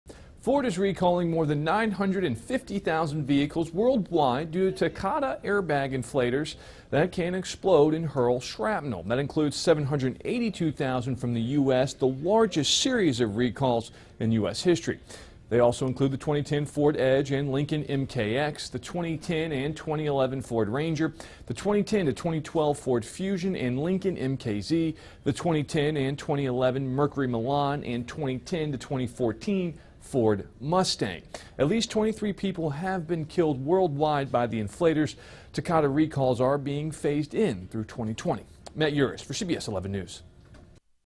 Ford Recalls Over 953,000 Vehicles To Replace Air Bag Inflators
Ford has issued a worldwide recall of more than 953,000 vehicles to replace Takata passenger air bag inflators that can explode and hurl shrapnel. Matt Yurus reports.